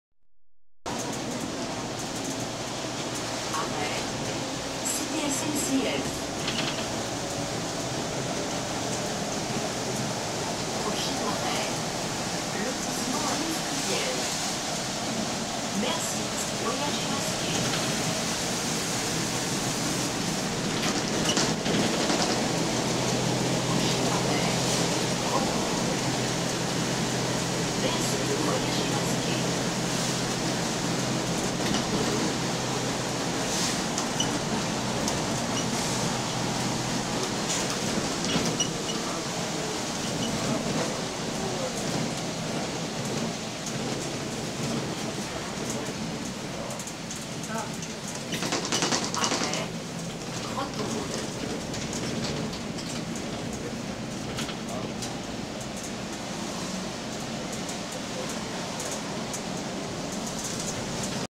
A 80 km/h avec le Citaro C2 1833
Vidéo faite le 02 octobre 2020 a 13:47 sur le service 01-07 de la ligne L1